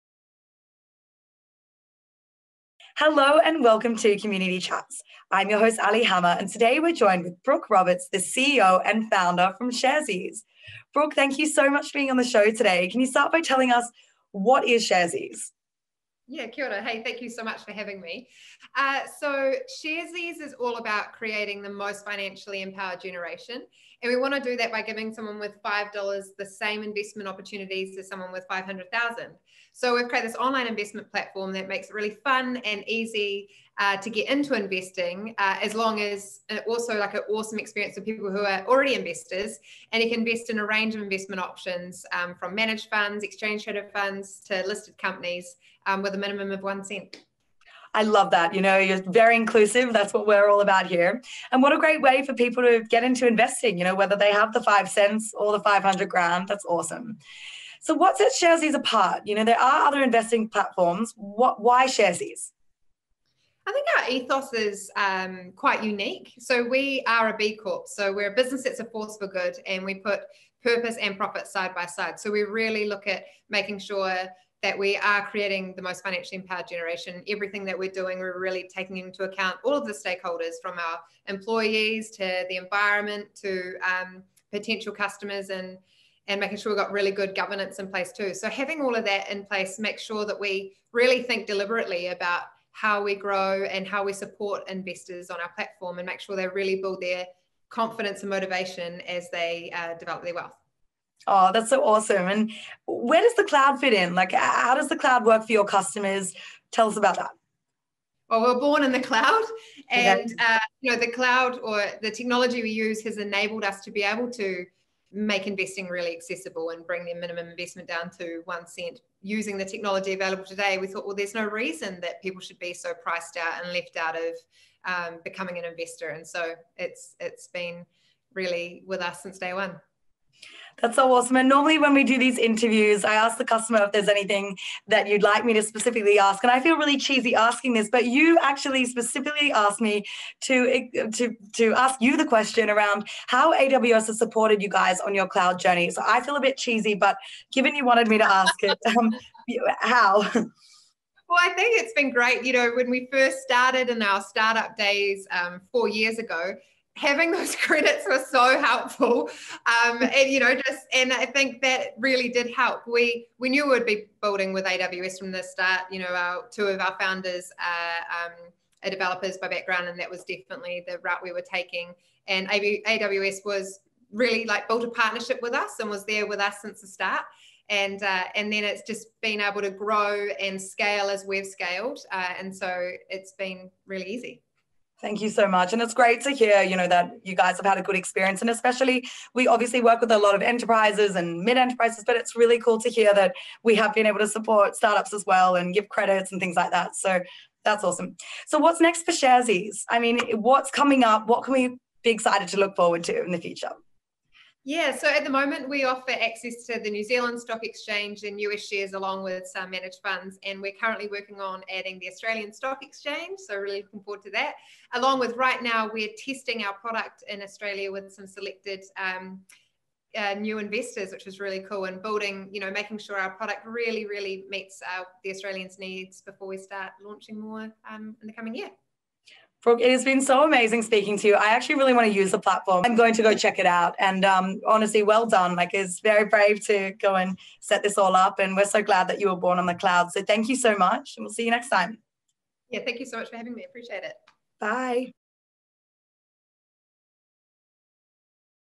Sharesies on AWS: Customer Story
In this episode of Community Chats, Aley Hammer, AWS Community Lead ANZ, interviews Brooke Roberts from Sharesies. Brooke explains what sets Sharesies apart from other investing services, how the company utilizes the AWS cloud and how AWS has supported Sharesies during their cloud journey.